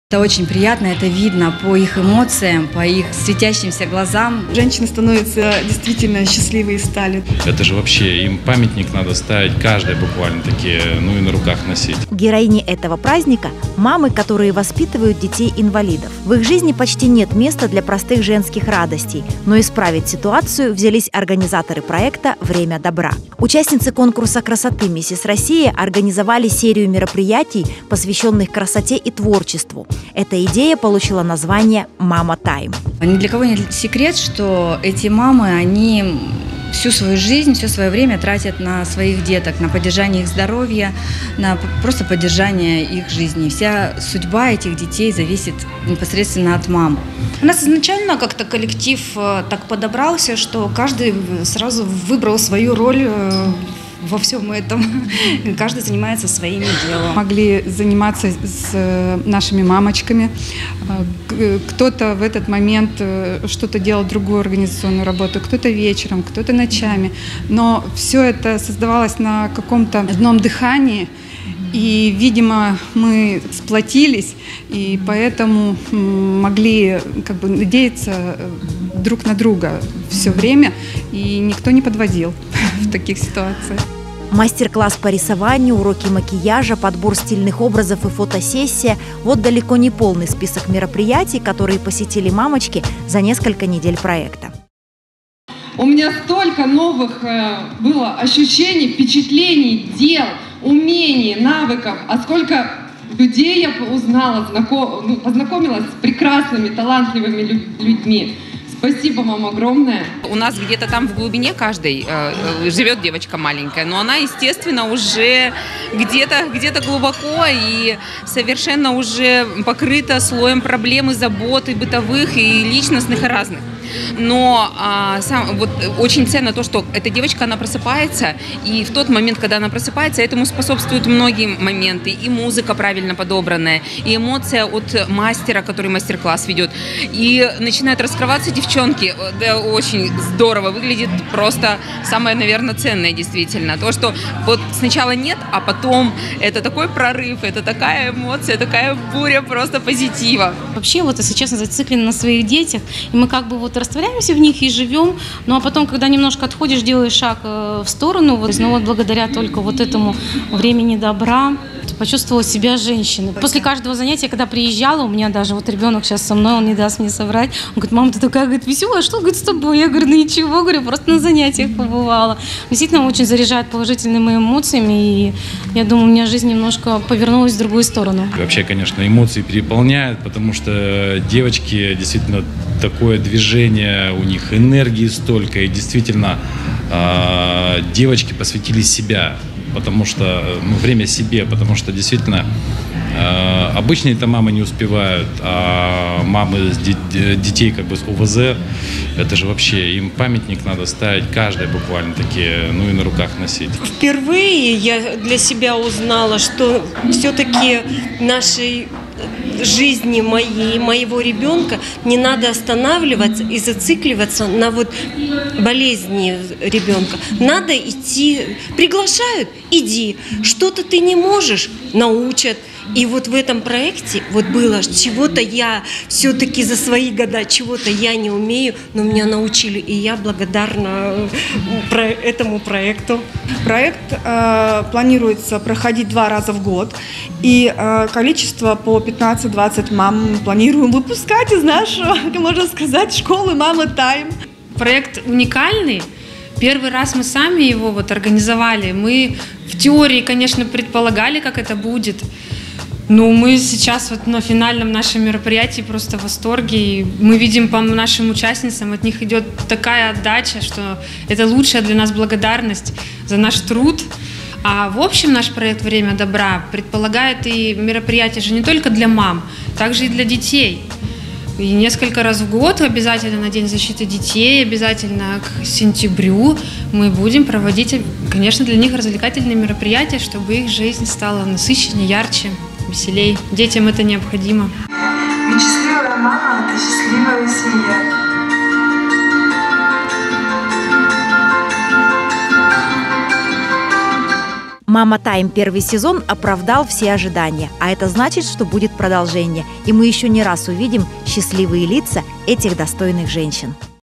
MAMA TIME- проект возрождающий женщин
Лишь благодаря поступкам людей город становится красивым. И нам, хочется рассказать о проекте MAMA TIME. Не для кого не секрет, что мамы, которые воспитывают детей с ОВЗ полностью посвещают свое время для ребенка, а на себя не остается ни минуты. Поэтому, участницы конкурса - Миссис Россия Южный Урал придумали проект, который вновь помог мамочкам обрести свою женскую натуру.